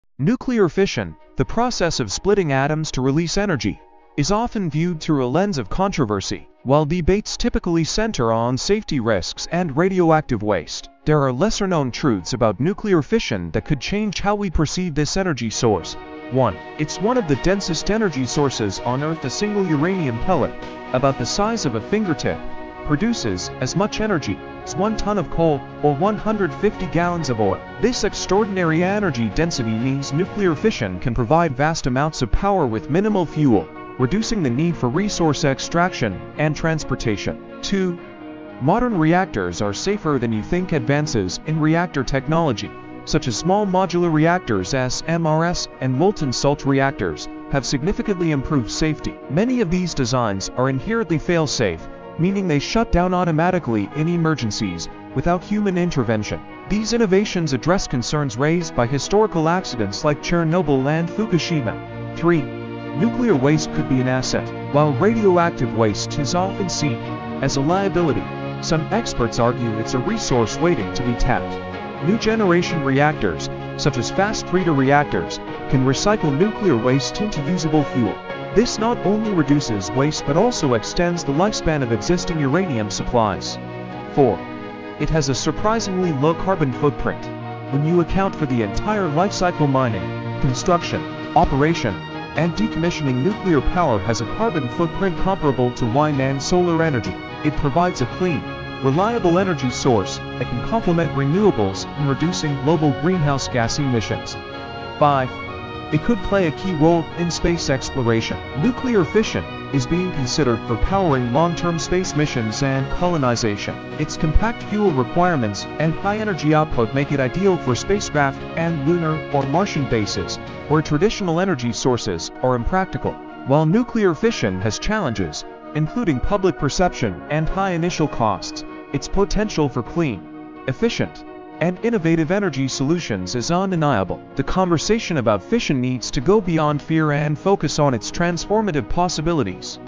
The Surprising Truth About Nuclear Fission Energy Nobody Tells You
Is nuclear fission the energy solution we’ve been overlooking? In this video, we uncover the surprising truths about nuclear fission energy that most people don’t know. From its incredible potential for clean energy to the challenges of waste management and safety, we break down the pros, cons, and myths surrounding this controversial power source. Discover what could make nuclear fission a game-changer for our planet’s energy future!